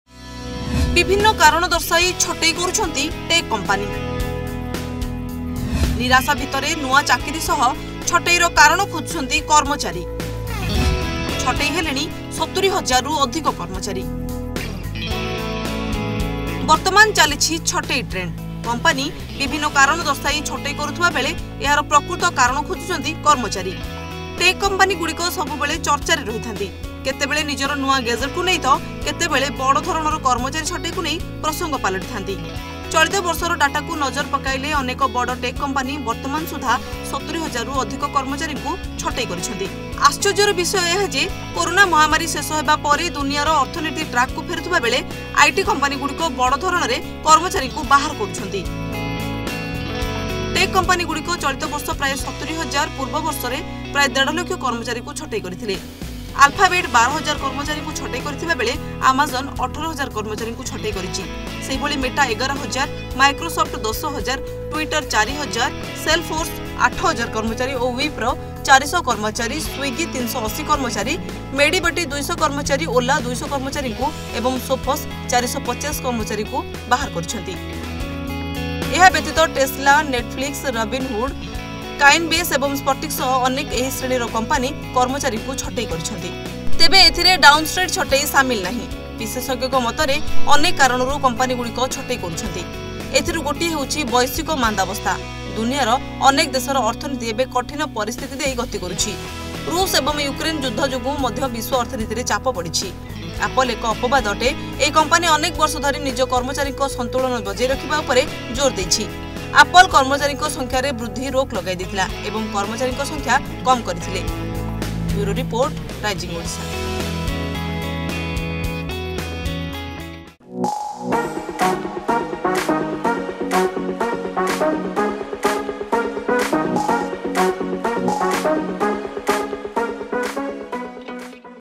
Why Tech Companies Are Firing Employees | More Than 70,000 Employees Sacked | Big Tech Layoffs 2023
More than 70000 Employees Sacked this year in Big Tech Layoffs 2023.